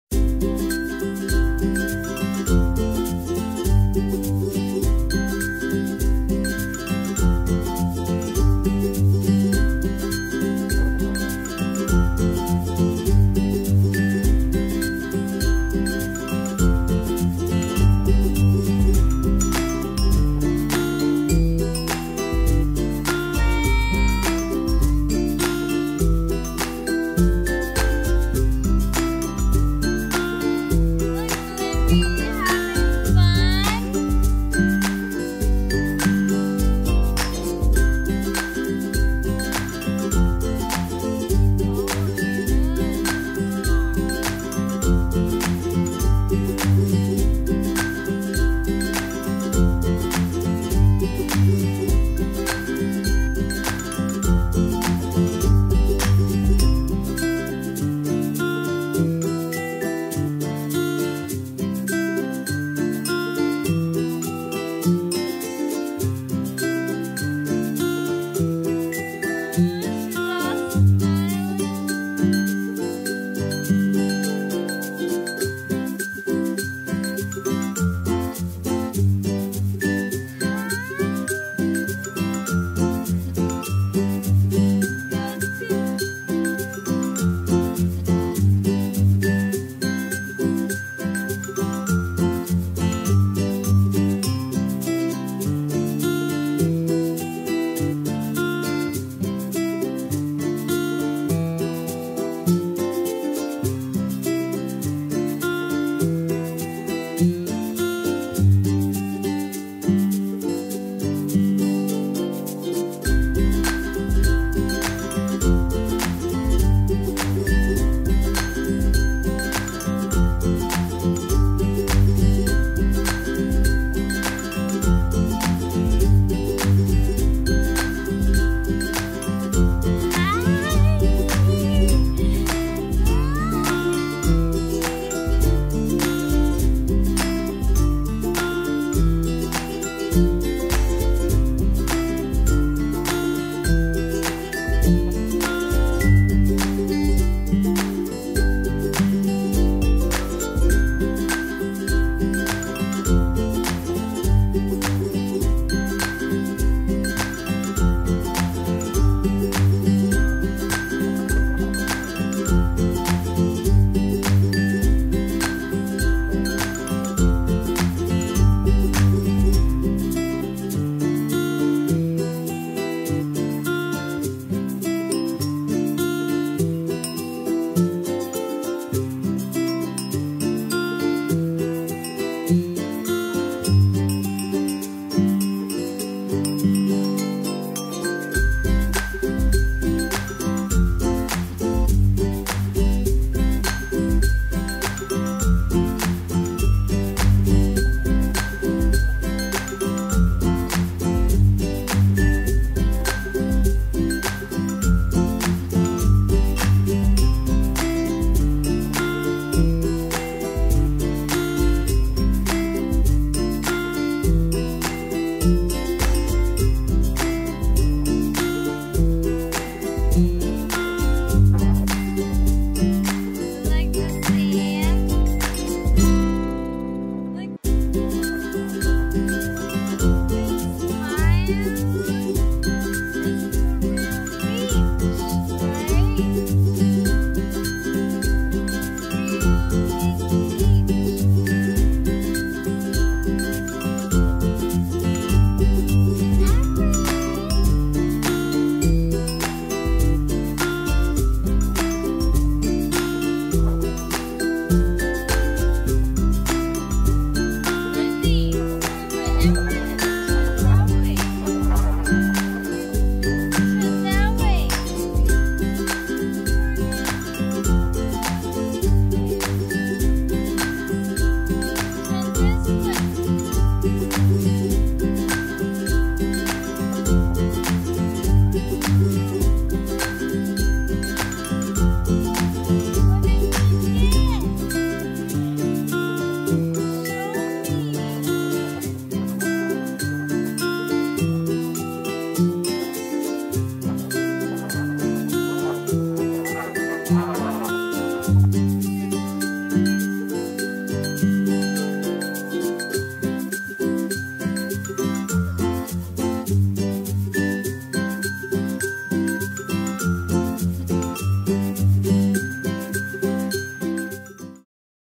Daytona Beach
Family vacation to Florida
Location: Daytona Beach, FL